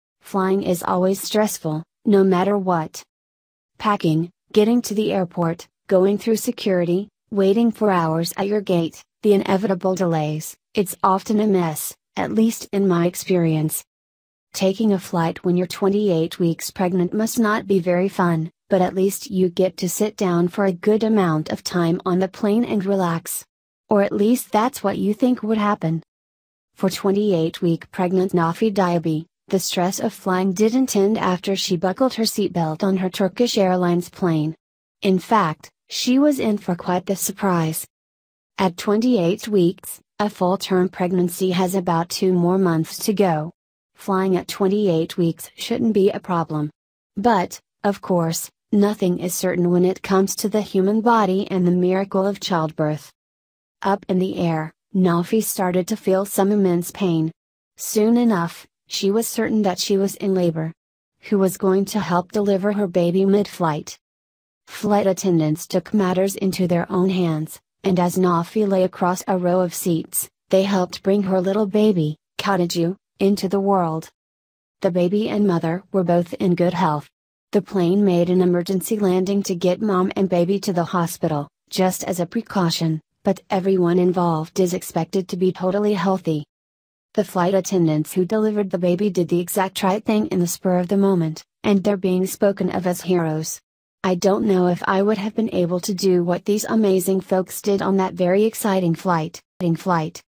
Flight Attendants Get A Massive Surprise When 28 Week Pregnant Woman Goes Into Labor Mid Air
Flying is always stressful, no matter what. Packing, getting to the airport, going through security, waiting for hours at your gate, the inevitable delays — it’s often a mess, at least in my experience! Taking a flight when you’re 28 weeks...